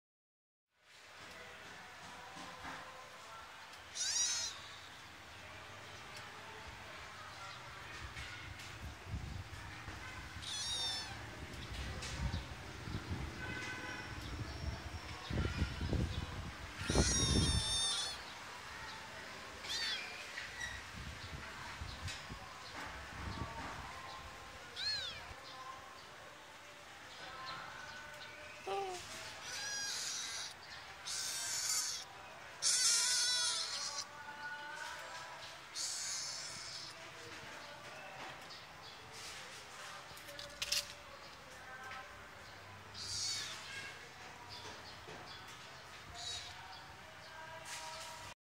A lovely and pity baby cat needs help
This lovely cat is leave home by her owner. And she just walked every where to find some food.